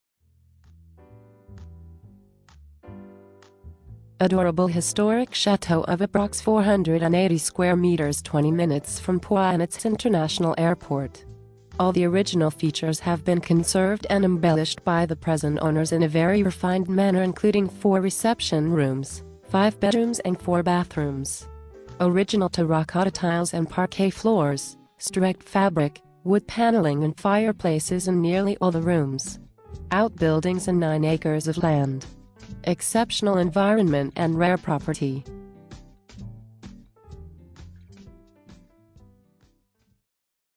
17th century chateau for sale in the béarn with an outstanding view of the Pyrenees
Adorable historic château of approx 480m² 20minutes  from Pau and its international airport. All the original features have been conserved and embellished by the present owners in a very rafined manner including 4 reception rooms, 5 bedrooms and 4 bathrooms. Original terracotta tiles and parquet floors, streched fabric,wood panelling and fireplaces in nearly all the rooms.Outbuildings and 9 acres of land. Exceptional environment and rare property